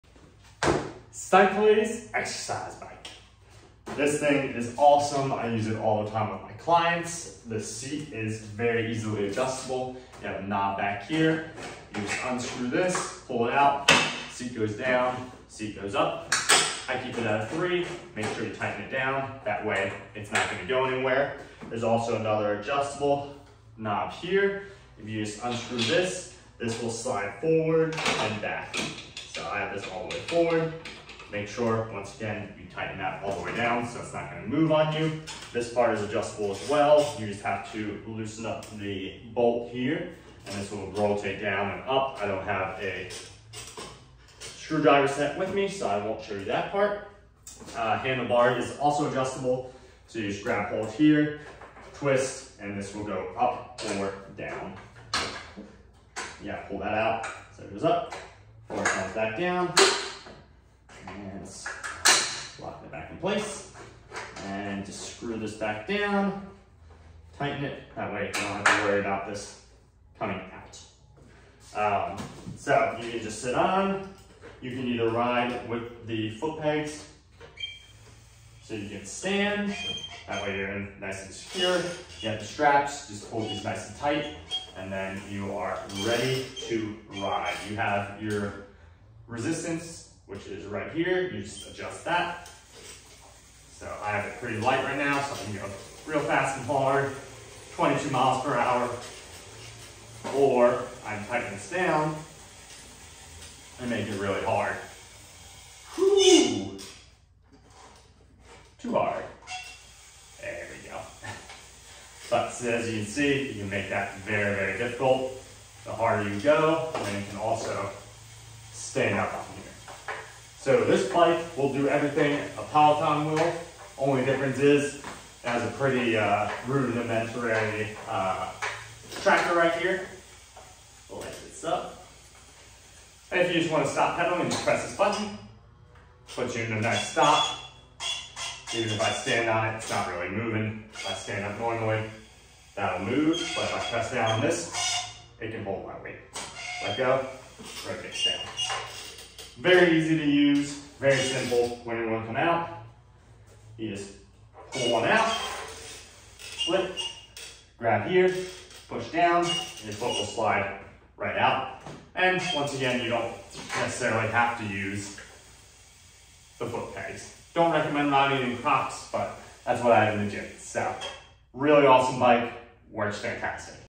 Using The Cyclace Exercise Bike! This Bike Is Great!
I Love This Stationary Bike!